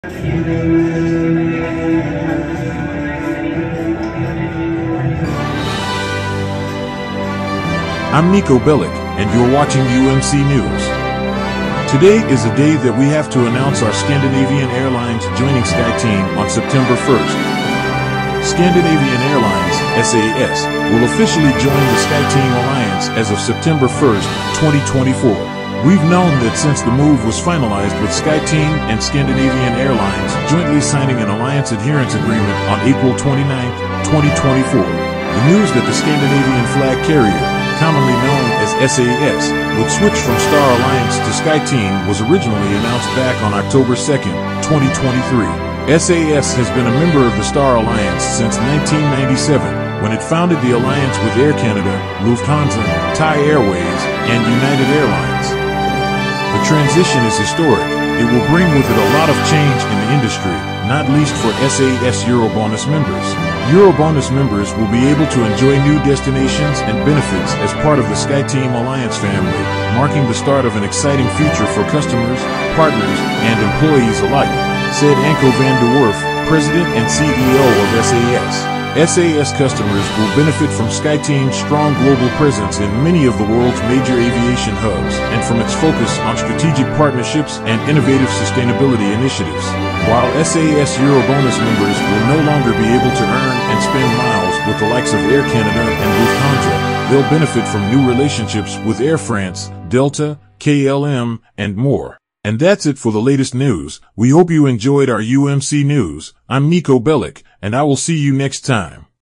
Scandinavian Airlines (SAS) Joining SkyTeam on September 1st | UMC News
Scandinavian Airlines (SAS) will officially join the SkyTeam Alliance as of September 1, 2024. We’ve known that since the move was finalized with SkyTeam and Scandinavian Airlines jointly signing an Alliance Adherence Agreement on April 29, 2024.